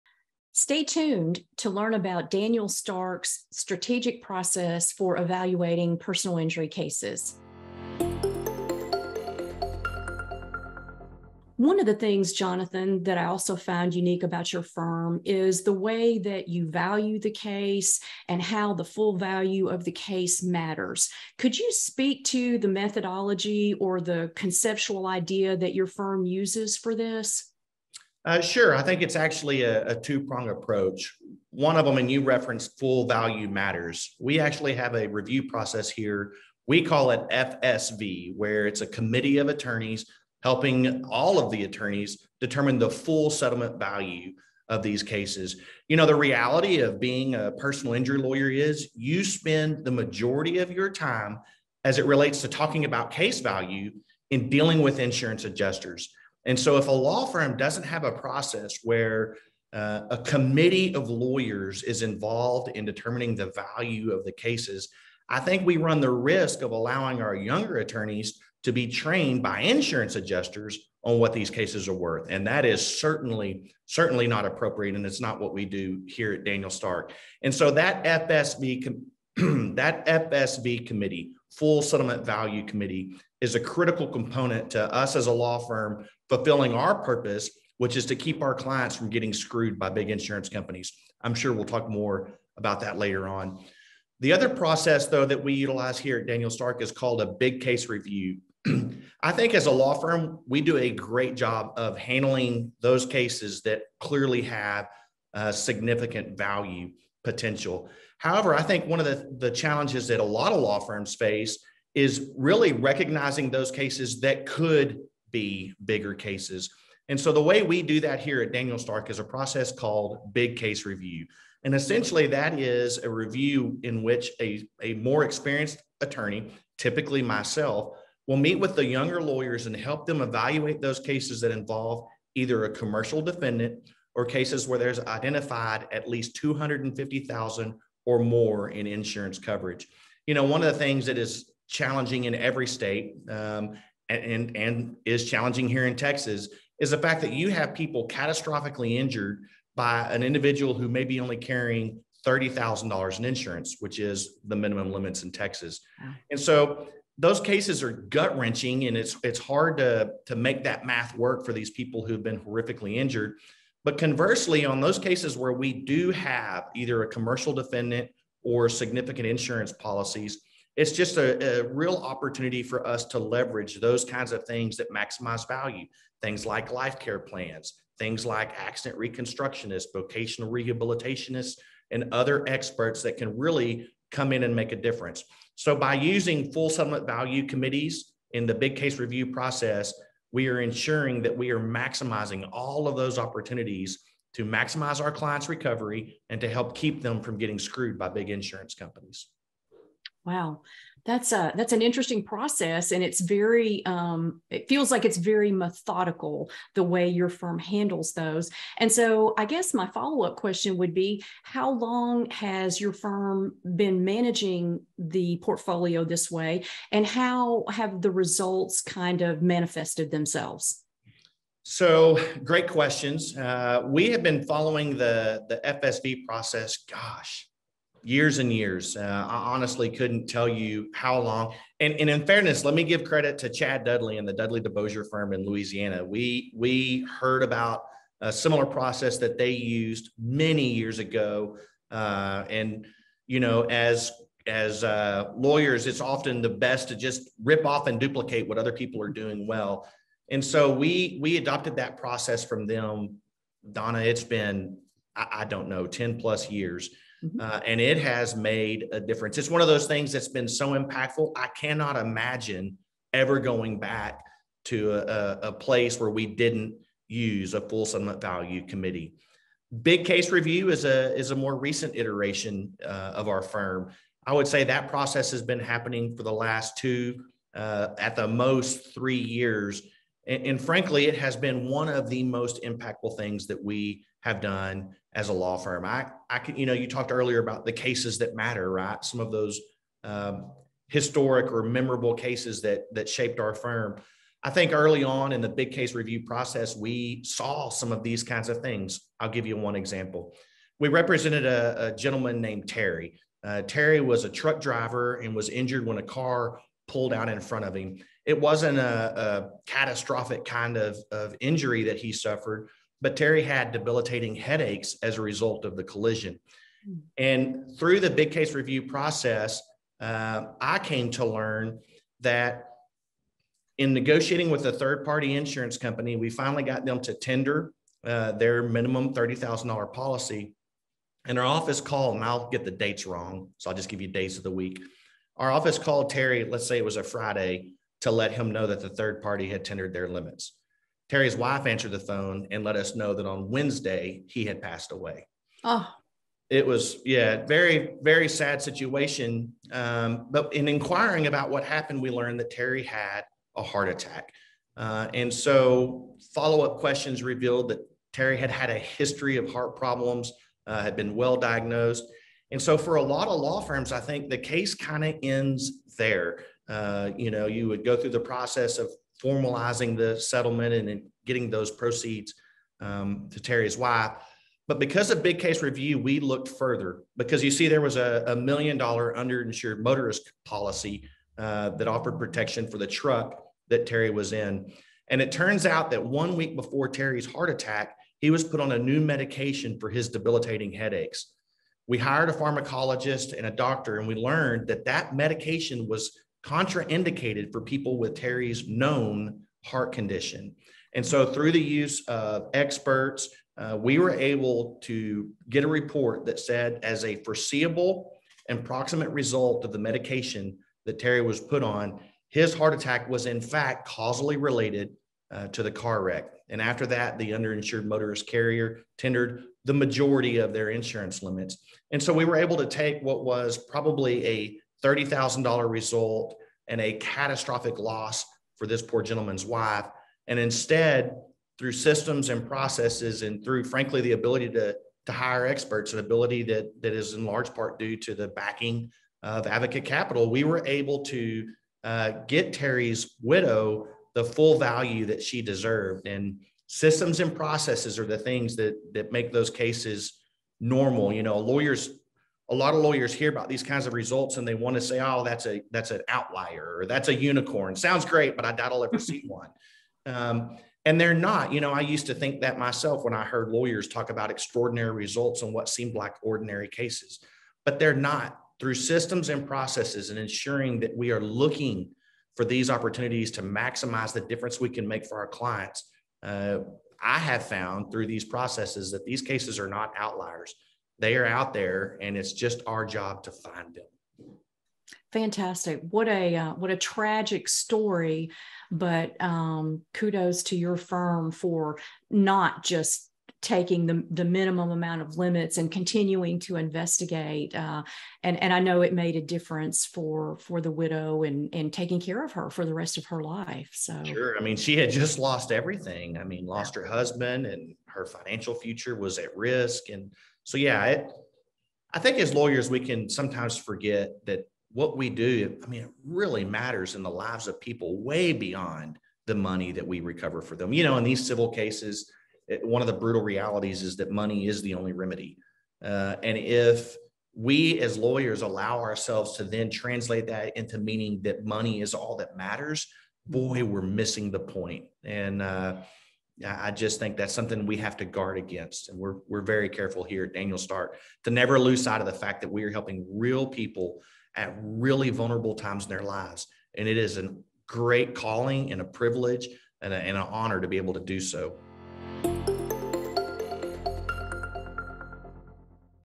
Strategic Processes for Evaluating Personal Injury Cases with Daniel Stark Injury Lawyers
Jonathan Stark was interviewed by Donna Jones, President, and Director of Associate and Client Experience, to discuss evaluating personal injury cases.